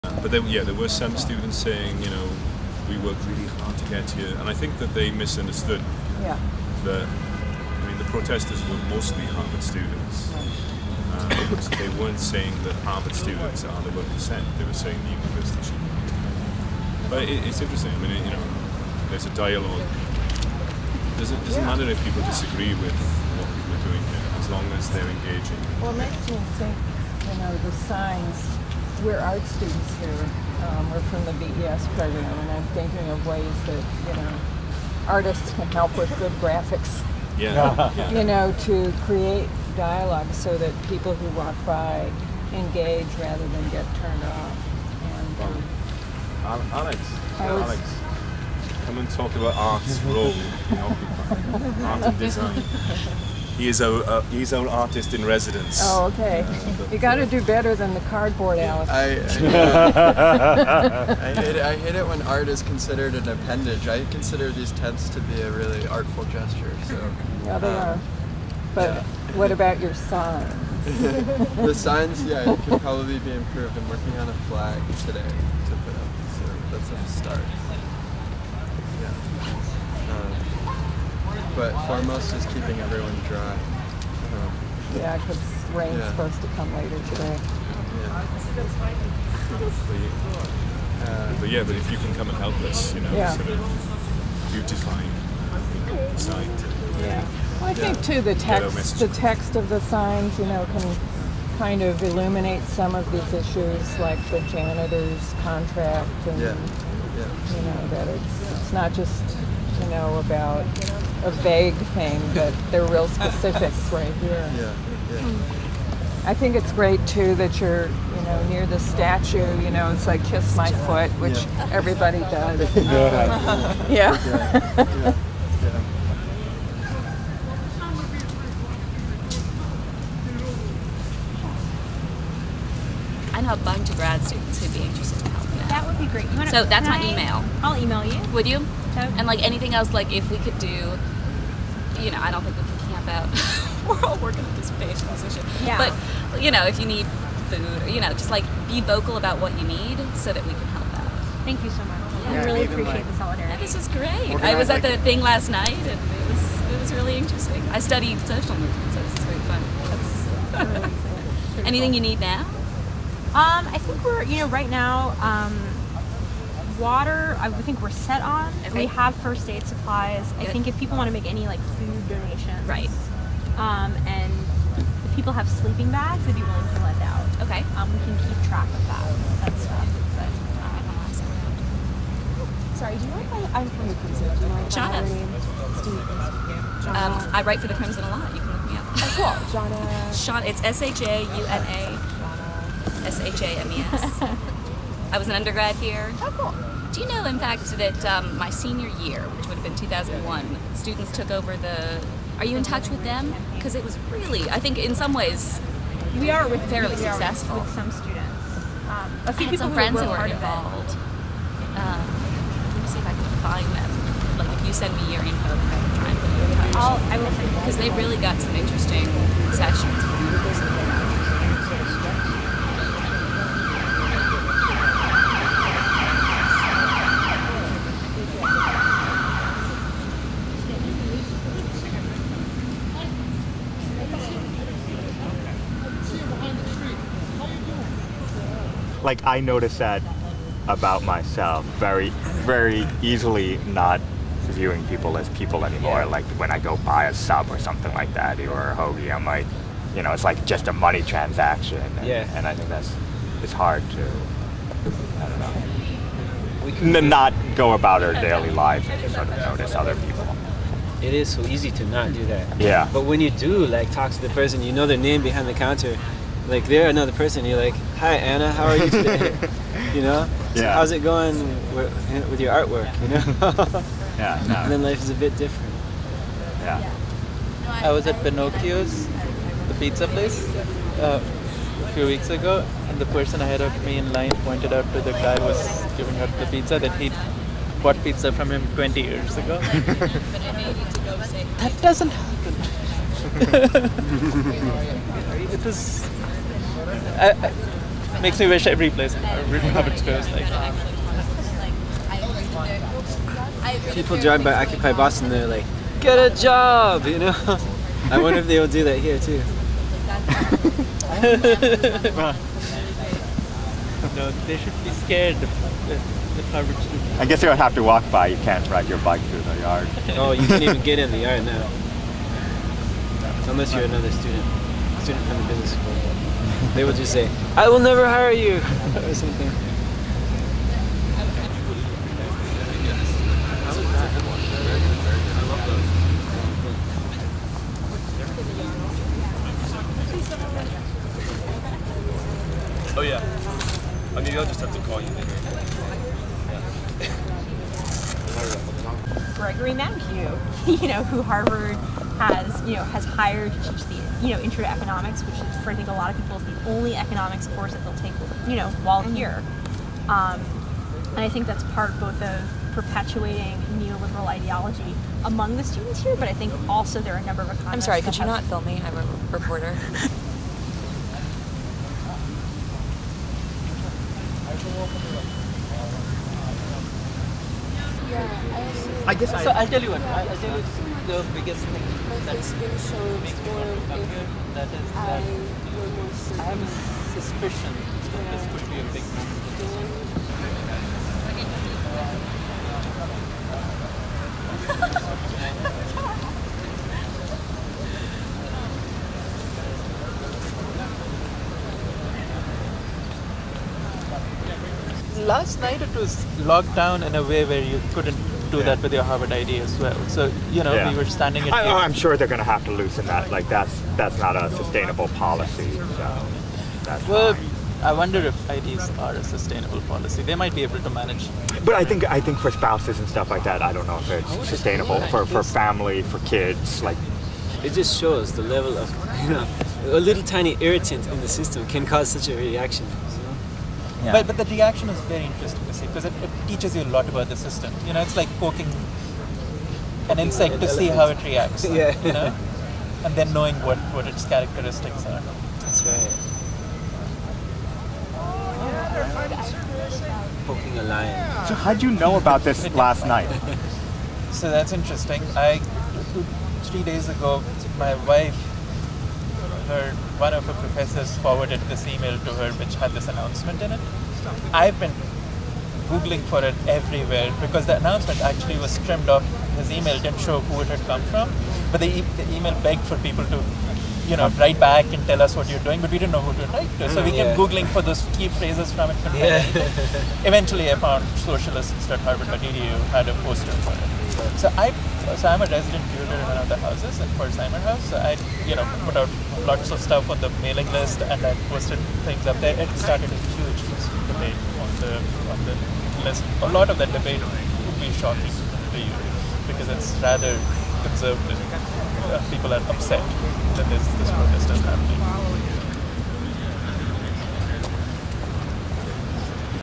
Ocupy Harvard Protest: 11:30am, November 11, 2011
Students meet in Harvard Yard to discuss their concerns about the global economy and the economic situation at their school.

Camera: Sony NEX-VG10